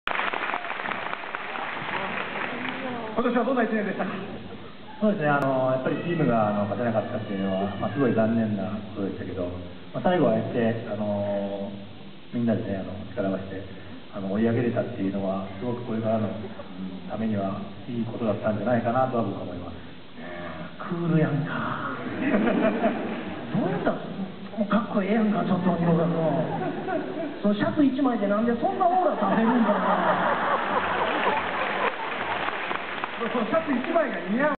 2010年12月22日 ガンちゃんと楽しい仲間たちによるチャリティーイベント⑧
二岡選手、今シーズンを振り返る